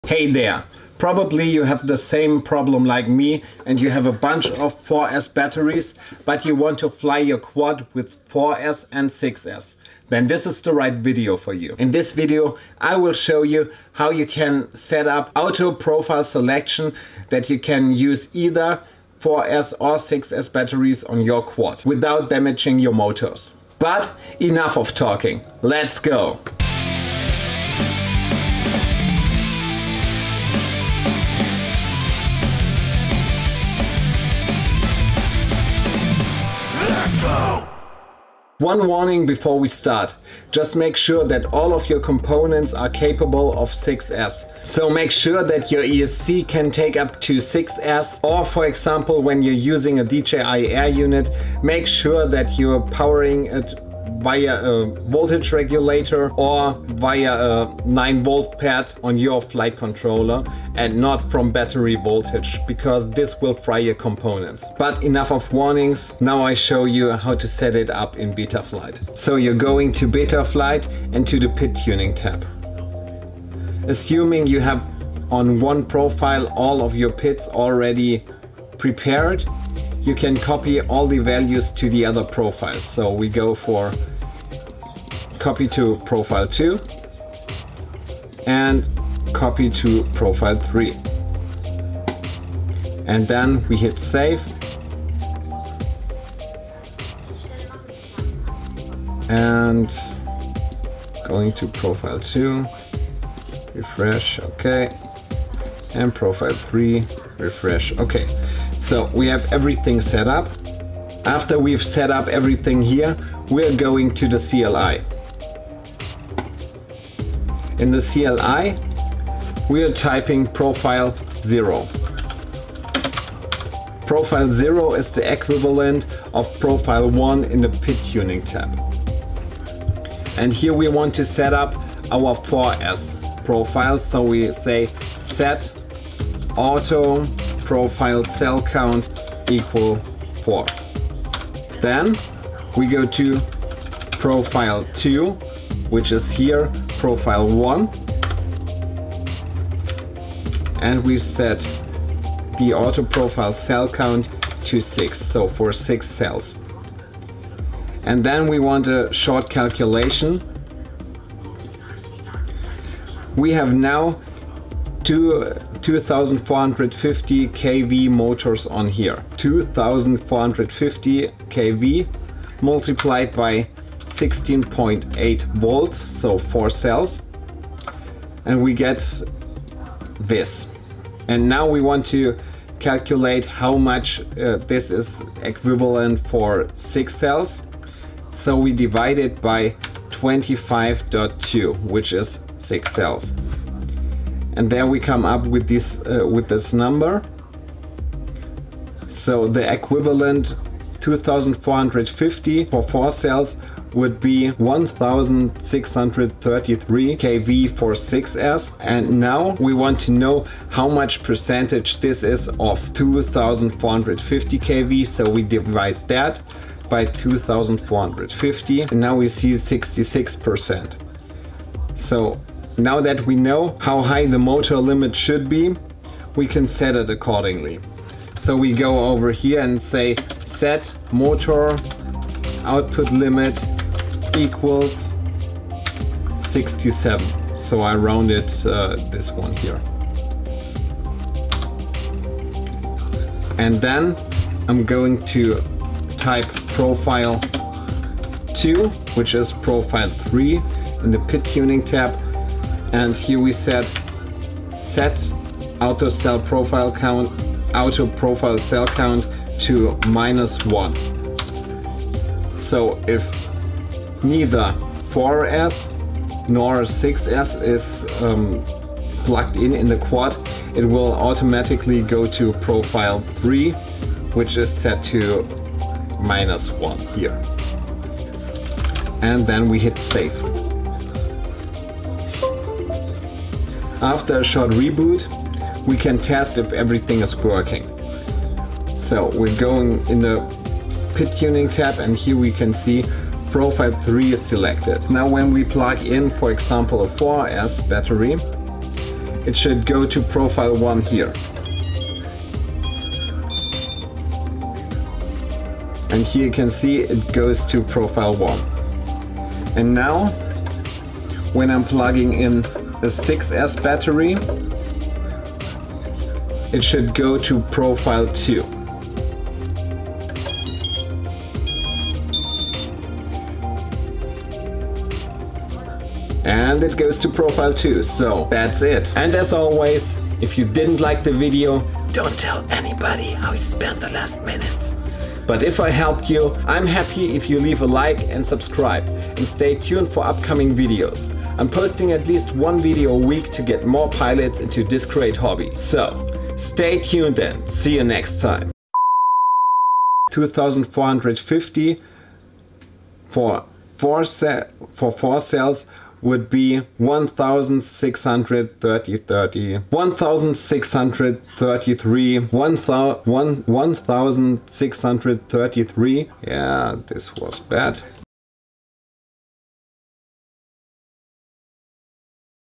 6S Batteries on 4S Motors - Setup Motor Output Limit And Auto Profile Selection
Probably you have a lot of 4S batteries, but you want to try or use 6S batteries as well? Then this is the right video for you!
In this video I show you how you can setup a motor output limit and the auto profile selection in Betaflight or Emuflight.
When you're following the steps then your quad will recognize which battery is plugged in and select the right PID profile automatically.

✦✦✦✦✦Chapters✦✦✦✦✦
0:00 Intro
0:38 Short Warning
1:06 Copy PID Profiles
1:56 Setup the CLI
4:43 Test
5:28 Outro
5:48 Outtakes